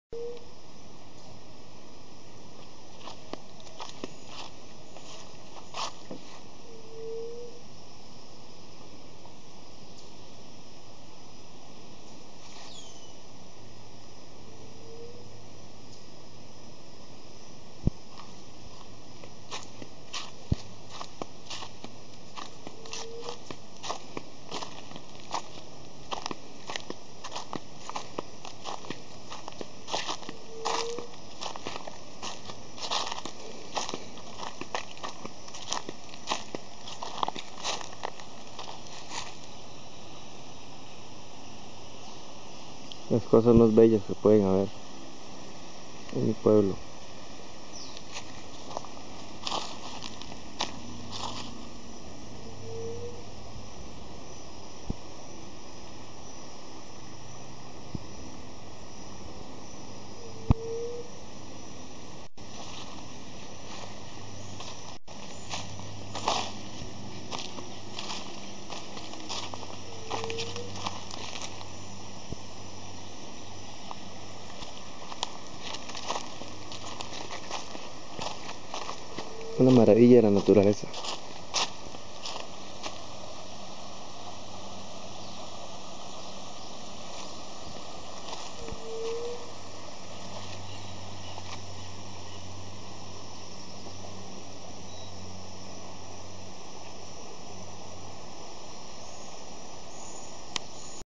Video de el río morote 3
Descripción Video filmado en el obispo de la Mansión de Nicoya Guanacaste Costa Rica.